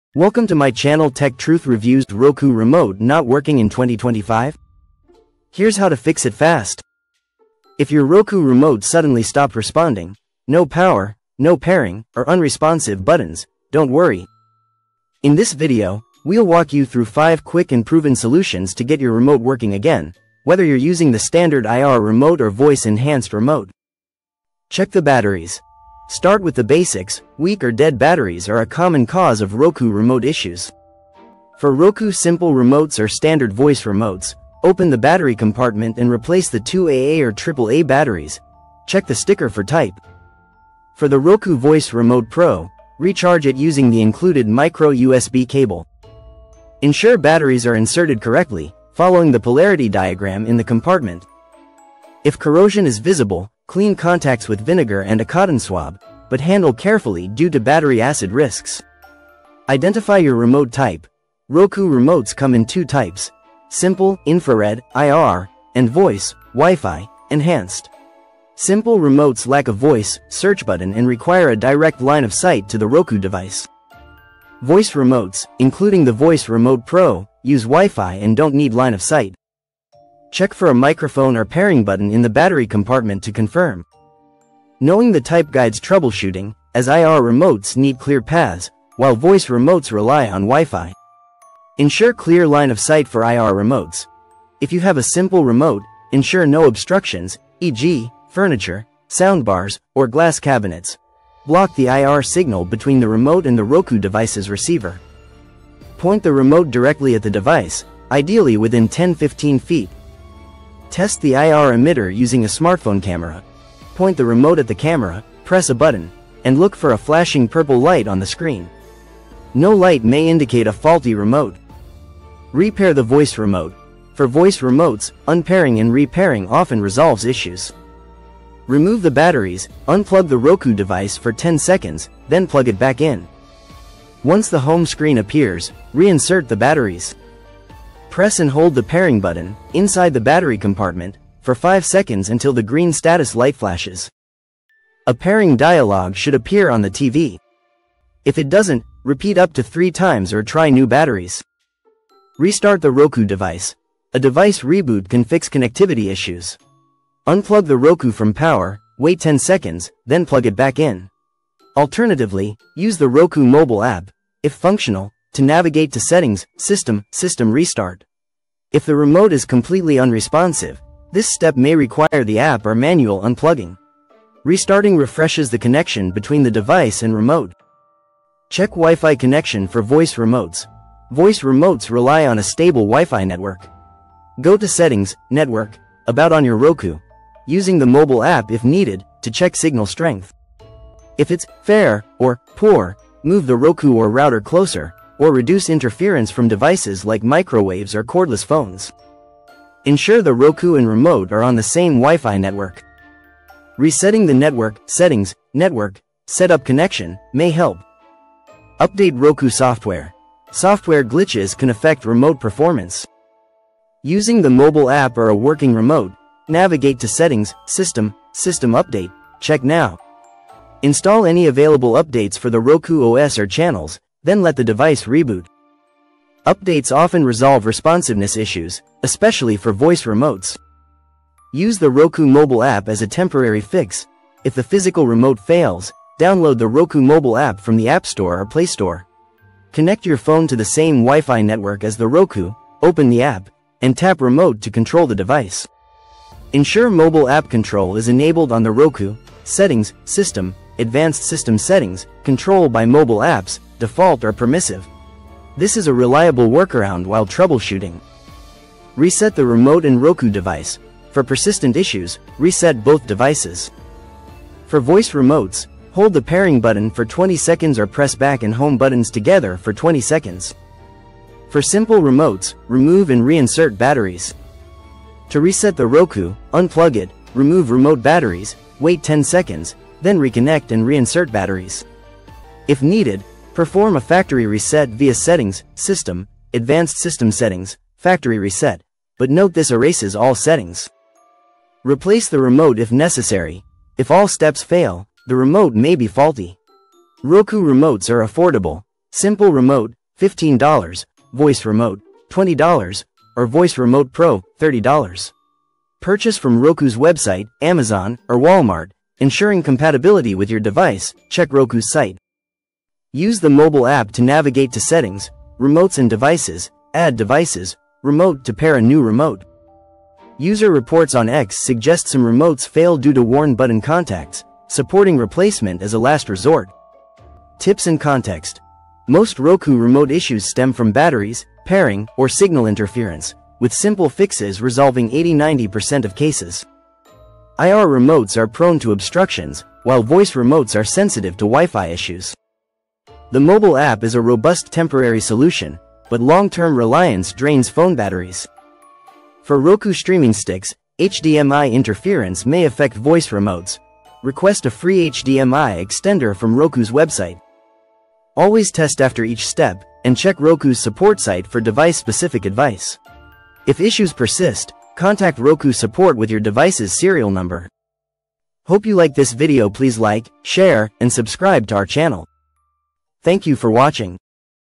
📺 Roku Remote Not Working in 2025? Here’s how to fix it fast!
📺 Is your Roku remote not working in 2025? Whether it’s unresponsive buttons, pairing issues, or no power at all — this guide has you covered!

In this video, we walk you through step-by-step troubleshooting methods to fix any Roku remote — whether it's a Simple IR remote, Voice Remote, or Voice Remote Pro. Learn how to test, reset, pair, or replace your remote with ease.

✅ Covered in this video:
🔋 How to check and replace Roku remote batteries
📶 Pairing & re-pairing Voice Remotes
📱 Using the Roku mobile app as a backup
📡 Fixing IR remote signal issues
🌐 Checking Wi-Fi & Roku device connection
🔄 Restarting & resetting the Roku device
🛠️ Factory reset & replacement options